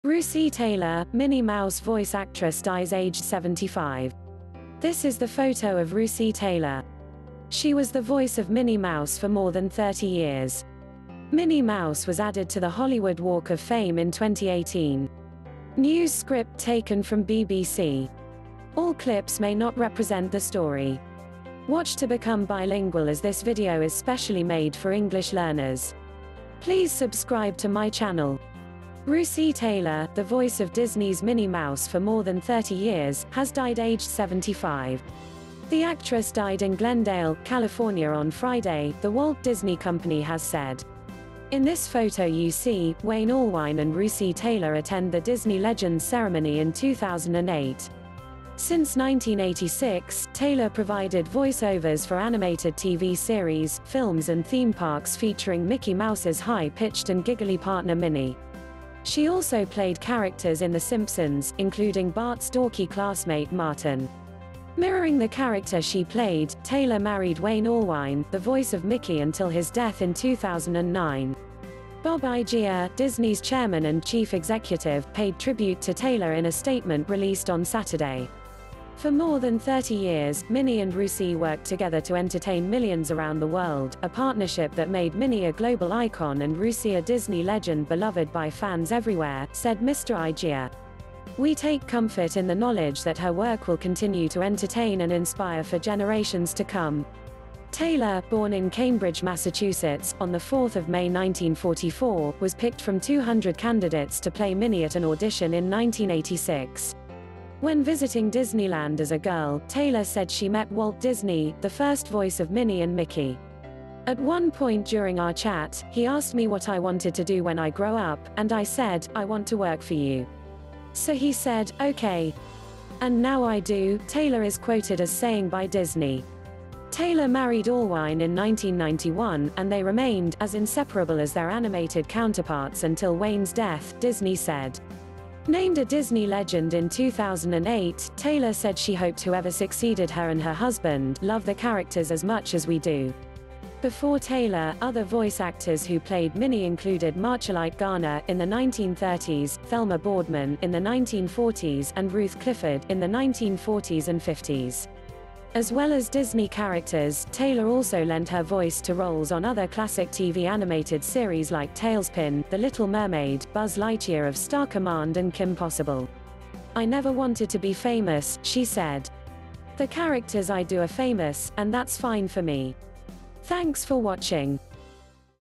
Disney Russi Taylor dies at 75 | Minnie mouse voice actress
Russi Taylor, the voice of Disney's Minnie Mouse for more than 30 years, has died aged 75.

This is the photo of Russi Taylor. She was the voice of Minnie Mouse for more than 30 years. Minnie Mouse was added to the Hollywood Walk of Fame in 2018.

The actress died in Glendale, California on Friday, the Walt Disney Company has said.

In this photo you see, Wayne Allwine and Russi Taylor attend the Disney Legends ceremony in 2008.

Since 1986, Taylor provided voice-overs for animated TV series, films and theme parks featuring Mickey Mouse's high-pitched and giggly partner Minnie.

She also played characters in The Simpsons, including Bart's dorky classmate Martin.

Mirroring the character she played, Taylor married Wayne Allwine, the voice of Mickey until his death in 2009.

Bob Iger, Disney's chairman and chief executive, paid tribute to Taylor in a statement released on Saturday.

"For more than 30 years, Minnie and Russi worked together to entertain millions around the world - a partnership that made Minnie a global icon and Russi a Disney Legend beloved by fans everywhere," said Mr Iger.

"We take comfort in the knowledge that her work will continue to entertain and inspire for generations to come."

Taylor, born in Cambridge, Massachusetts, on 4 May 1944, was picked from 200 candidates to play Minnie at an audition in 1986.

When visiting Disneyland as a girl, Taylor said she met Walt Disney - the first voice of Minnie and Mickey.

"At one point during our chat, he asked me what I wanted to do when I grow up, and I said, 'I want to work for you!' So he said, 'Okay!' And now I do!," Taylor is quoted as saying by Disney.

Taylor married Allwine in 1991, and they remained "as inseparable as their animated counterparts until Wayne's death", Disney said.

Named a Disney Legend in 2008, Taylor said she hoped whoever succeeded her and her husband "love the characters as much as we do".

Before Taylor, other voice actors who played Minnie included Marcellite Garner (in the 1930s), Thelma Boardman (in the 1940s) and Ruth Clifford (in the 1940s and 50s).

As well as Disney characters, Taylor also lent her voice to roles on other classic TV animated series like TaleSpin, The Little Mermaid, Buzz Lightyear of Star Command and Kim Possible.

"I never wanted to be famous," she said. "The characters I do are famous, and that's fine for me."

[News script taken from BBC. All clips may not represent the story. Watch to become bilingual as this video is specially made for English learners.